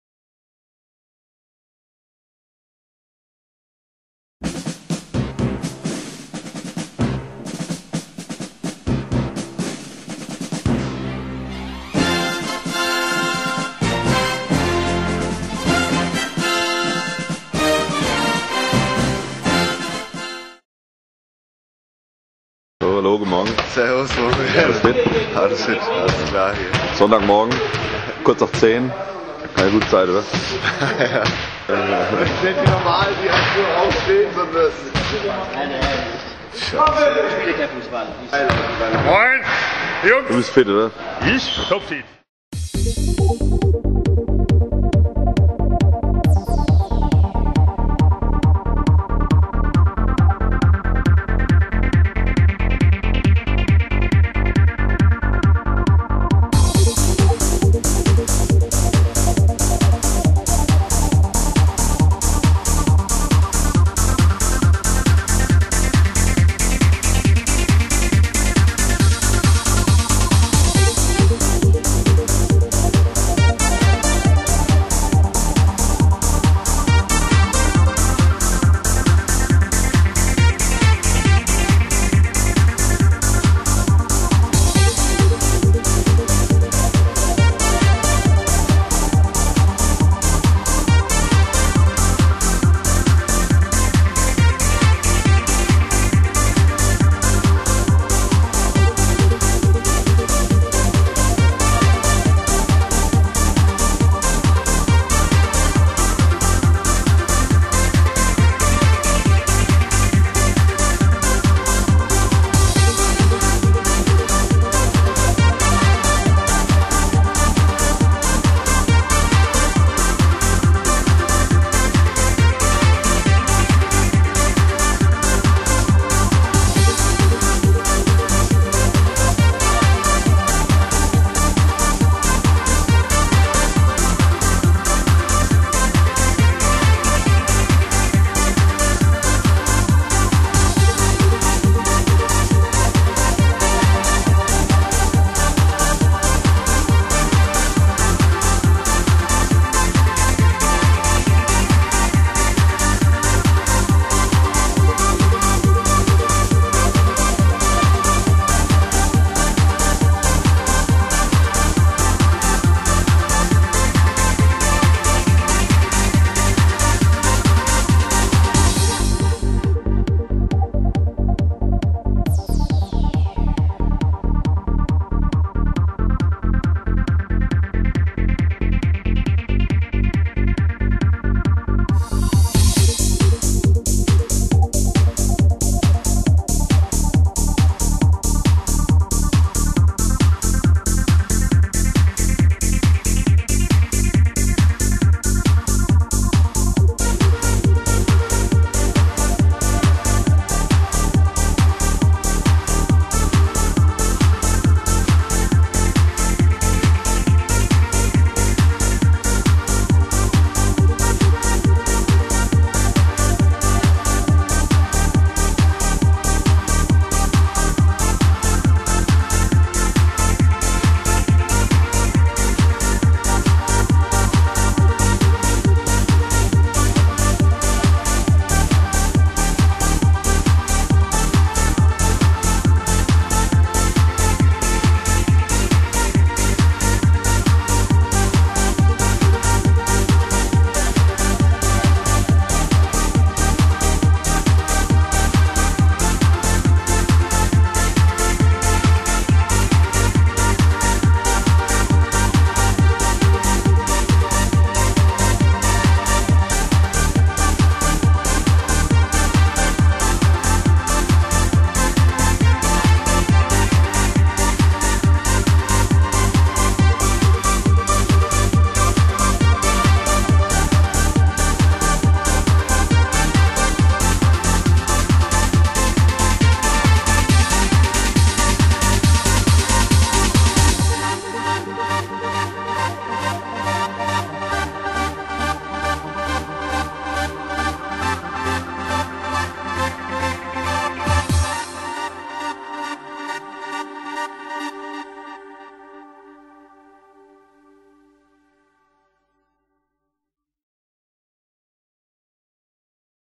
RGH Training mit Artur Sammet
Training mit der Rugbymannschaft des RGH Heidelberg